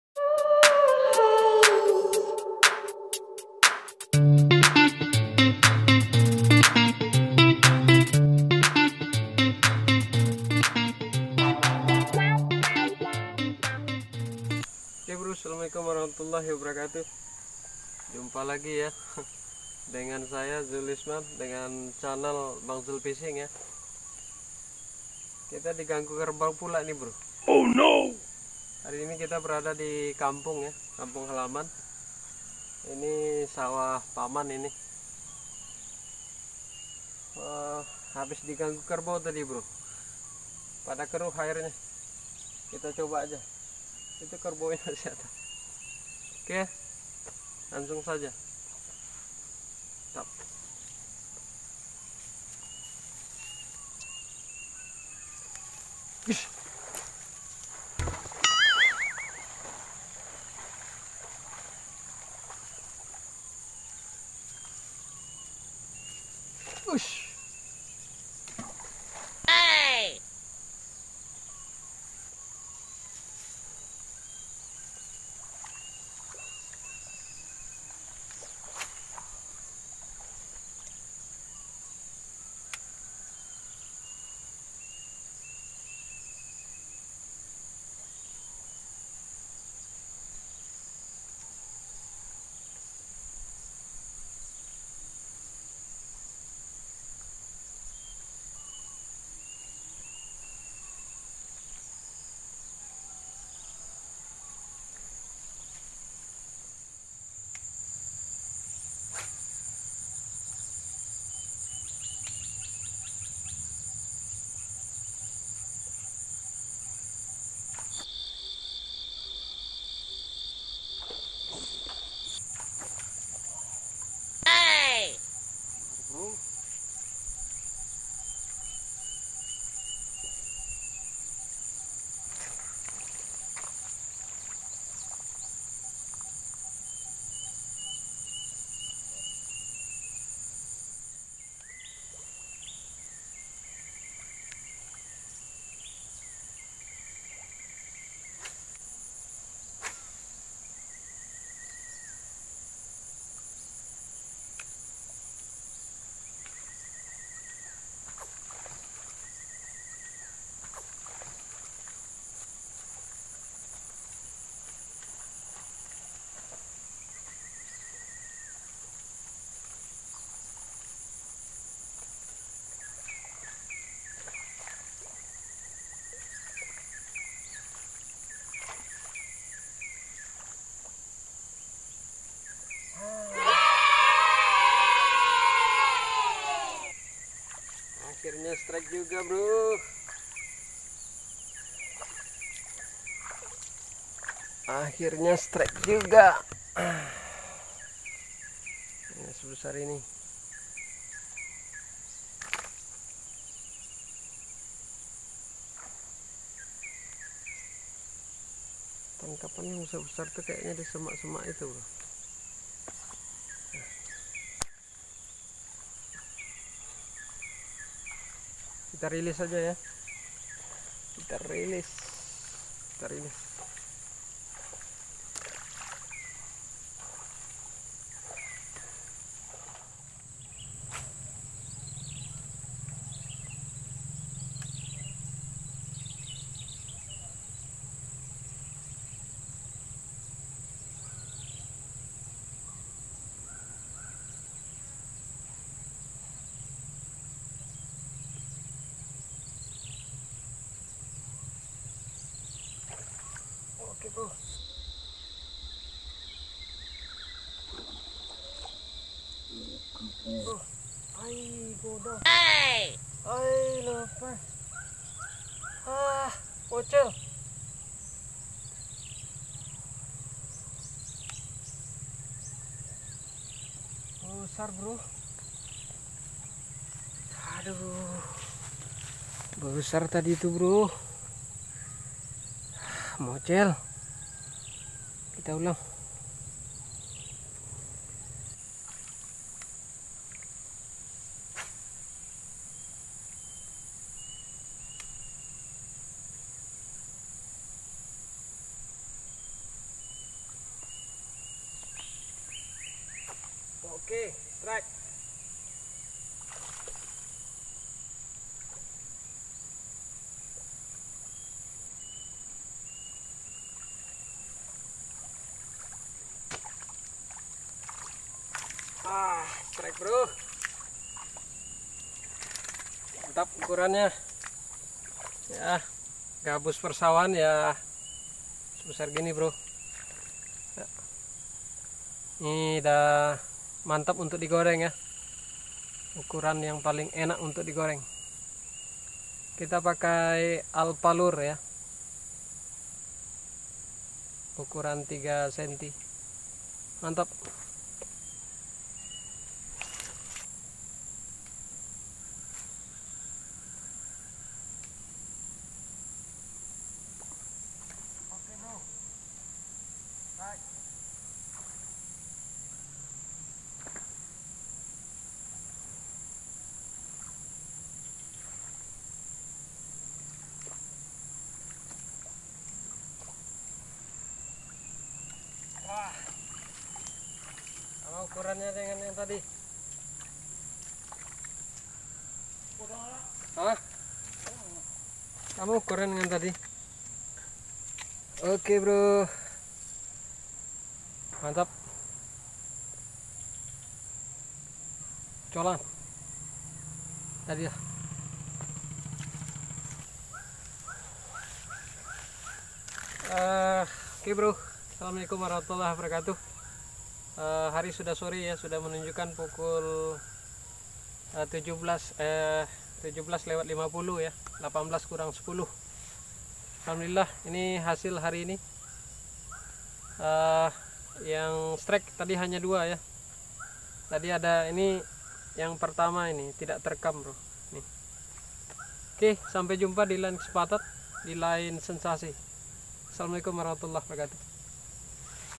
Sawah paman ini sarang Haruan || Casting Snakehead
Assalamualaikum sobat Angler semuanya...
Selamat berlebaran dan mohon maaf lahir dan batin...
Kali ini saya Casting di kampung halaman sobat, setiap saya pulkam...,sawah ini tak luput dikunjungi, karena memang haruan begitu nyaman berada di sini...
Bunyi letupannya banyak, terutama di semak2 rerumputan tersebut...
Penasaran saya belum tuntas di sini, karena babon2 ya belum pernah saya dapatkan.
Trims atas support sobat semua atas channel ini...